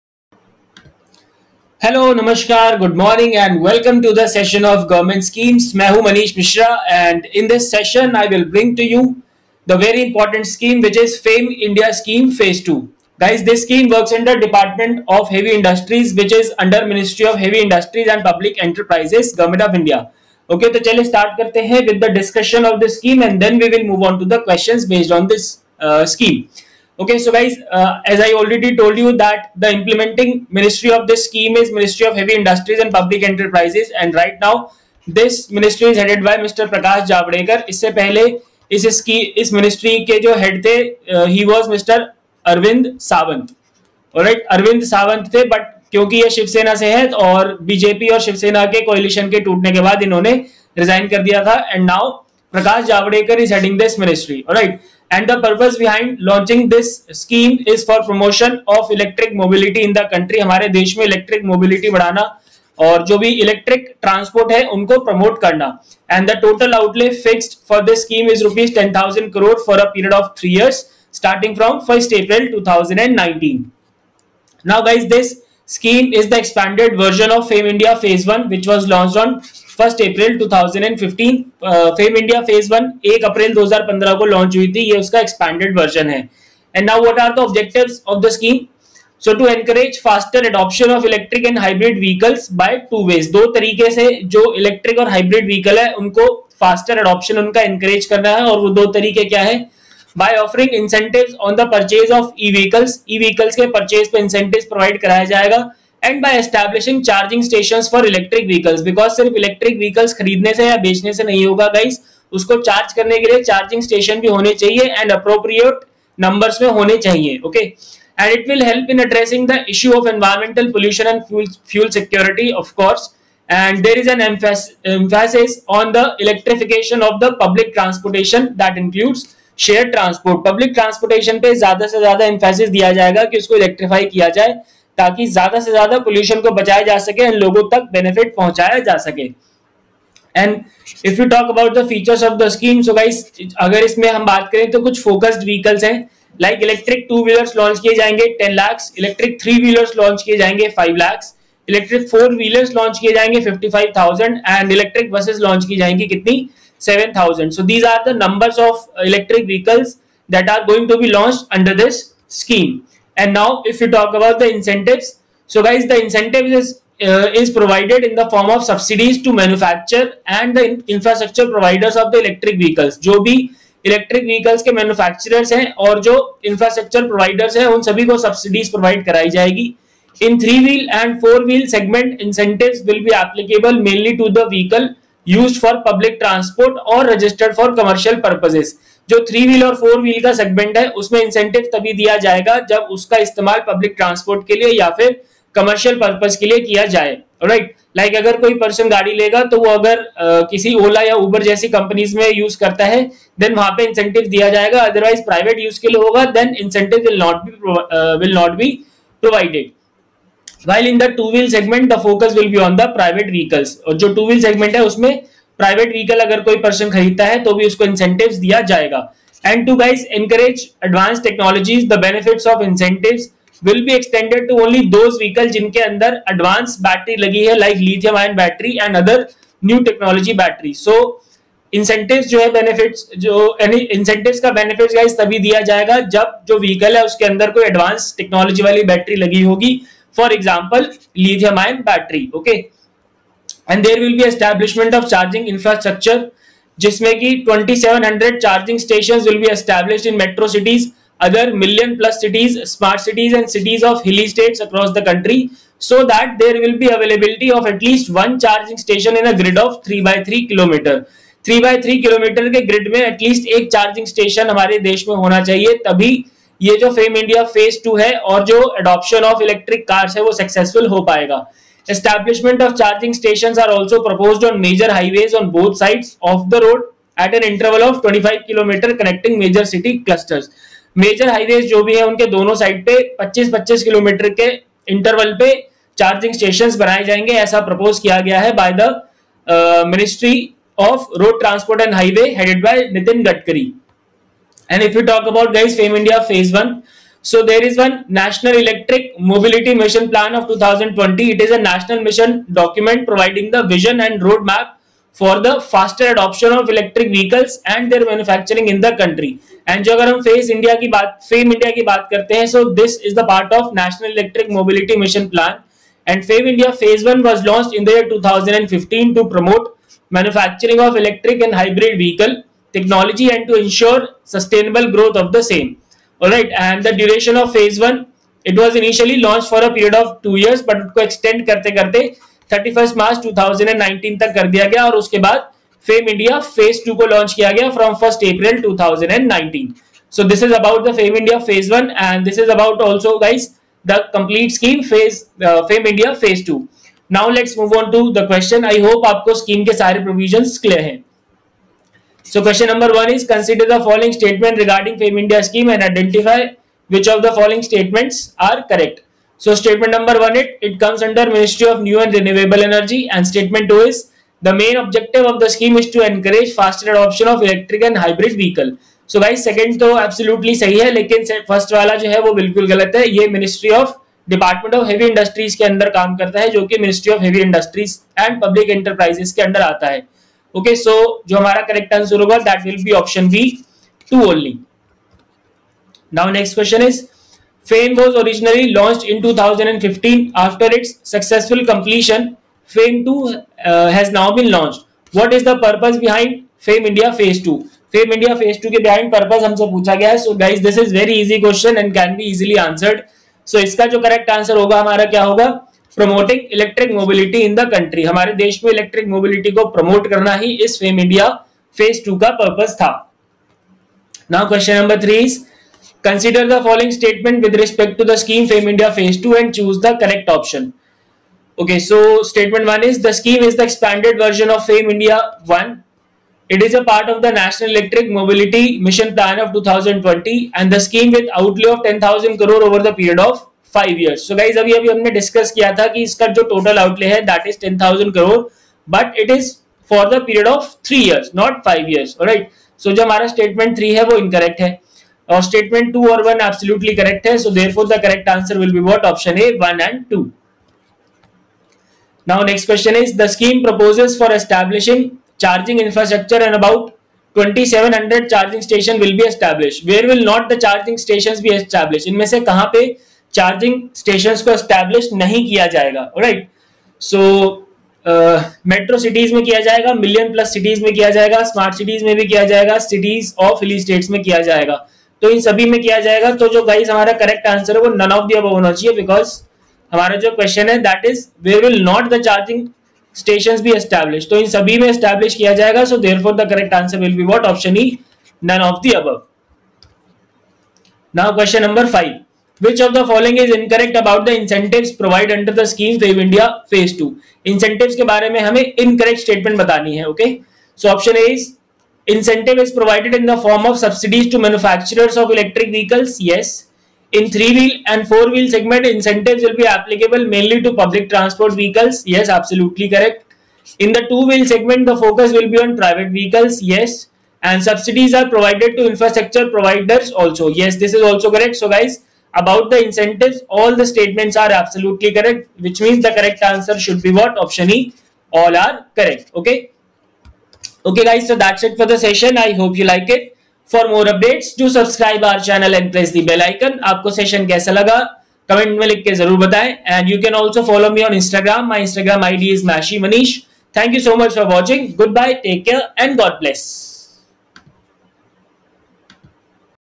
Important Government Schemes | Fame India Phase 2
In this session, we will discuss in detail all the provisions of the scheme FAME INDIA PHASE 2.
I will also bring you some important questions based on the same.
So watch the complete session and spread the knowledge!!